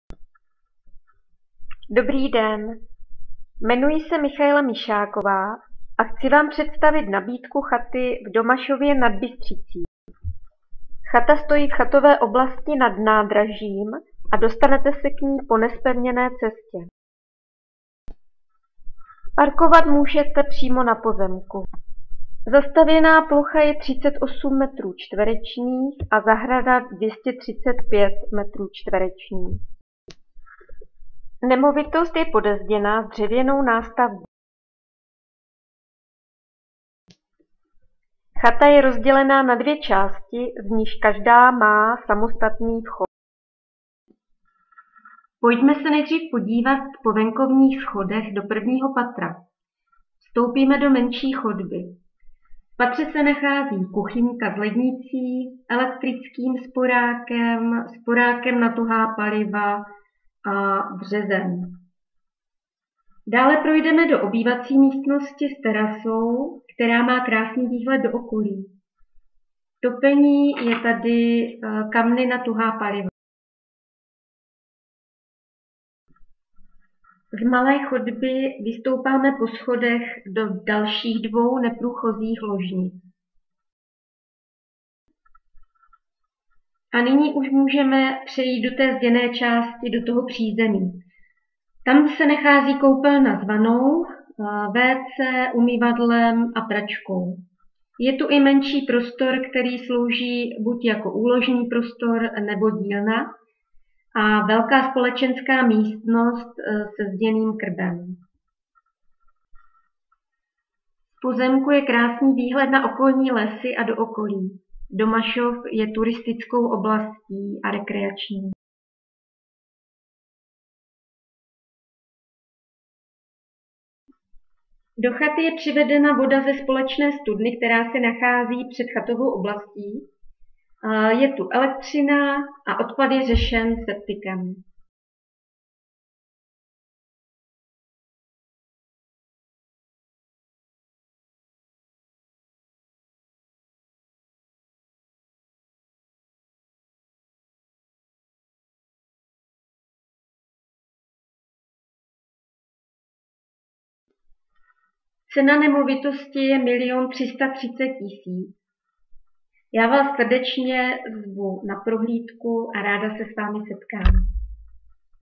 Prodej chaty v Domašově nad Bystřicí
Nabízím ke koupi chatu v obci Domašov nad Bystřicí. Chata se nachází v rekreační části nad vlakovým nádražím.
Zast. plocha je 38 m2 a zahrada 235 m2. Je podezděná, s dřevěnou nástavbou. Chata je rozdělená na dvě části se samostatnými vchody. 1.NP - vstup do zděné části, kde se nachází koupelna s vanou, WC, umyvadlem, pračkou, menší prostor, který slouží jako úložný nebo dílna a společenská místnost se zděným krbem.
2.NP - vstup po venkovních schodech do dalšího patra, kde se nachází kuchyňka s malým elektrickým sporákem, kamny na TP, lednicí, umyvadlem, obývací místnost s terasou a krbovými kamny a komora se vstupem do 3.NP, kde jsou dvě ložnice.
Do chaty je přivedena voda ze společné studny, elektřina a odpad je řešen septikem.
Příjezd je po nezpevněné cestě.
Z pozemku je krásný výhled do okolí. V okolí lesy, turistické trasy a cyklistické vyžití, lyžařský vlek, nejbližší koupání letní je v Dalově.
Obec se nachází asi 10 km východně od Šternberka, 28 km od Olomouce. Leží v Nízkém Jeseníku v zalesněném údolí říčky Bystřice, podél které také vede železniční trať z Olomouce do Opavy. V obci je praktický lékař, Mateřská škola, prodejna potravin, hostinec, pošta.